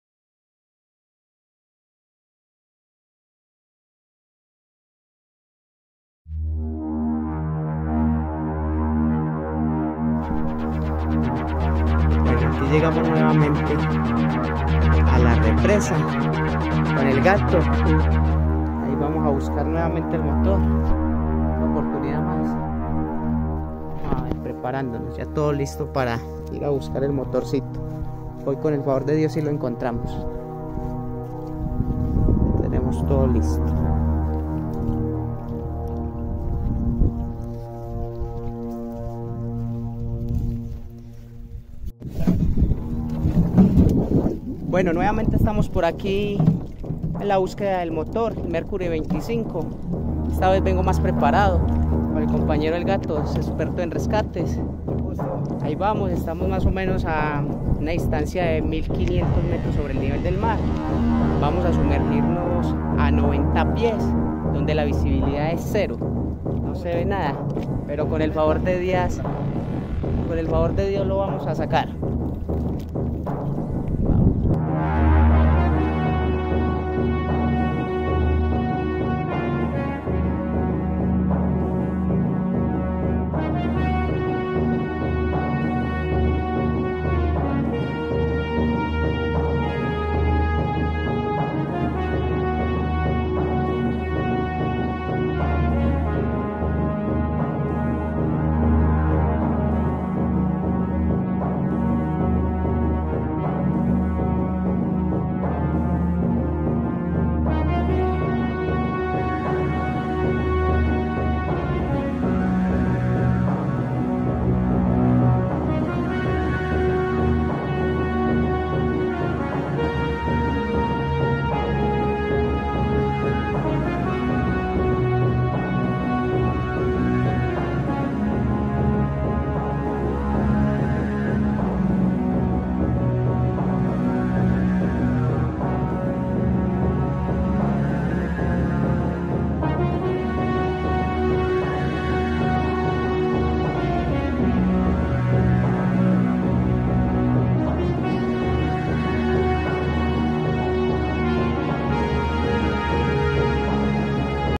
Rescate de motor ,primer intento
Created by InShot está vez con la compañía del señor jhon Alexander , profundidad lograda en el embalse de aguadas Antioquia 81 pies ,en la represa de guatape 64 ,pasamos de 1500 metros sobre el nivel del mar a 1870 ,mucho tráfico marino en guatape,estaba de noche ,fue peligrosa la inmersión,pero por fortuna por acá estamos de nuevo y preparando nuevas aventuras